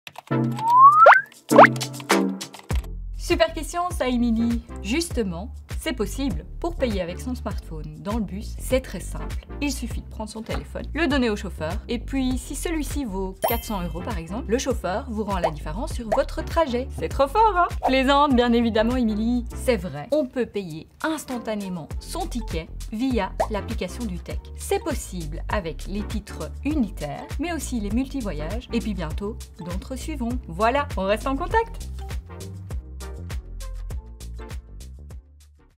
Pourquoi on ne peut pas acheter son titre avec son smartphone ?
🤩 C’est dingue tout ce qu’on peut faire avec un smartphone… Paraît qu’on peut même payer son titre de transport ! 🤔